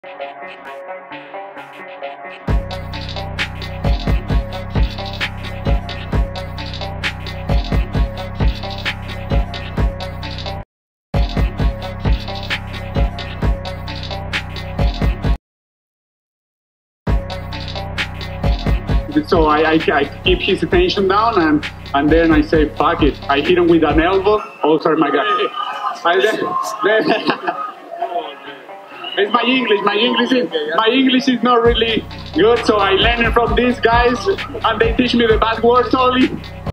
Ignacio Bahamondes stunning wheel kick KO with seconds left
Ignacio Bahamondes stunning wheel kick KO of Roosevelt Roberts with seconds left in the third and final round of a bout at UFC Fight Night on Aug. 21, 2021 in Las Vegas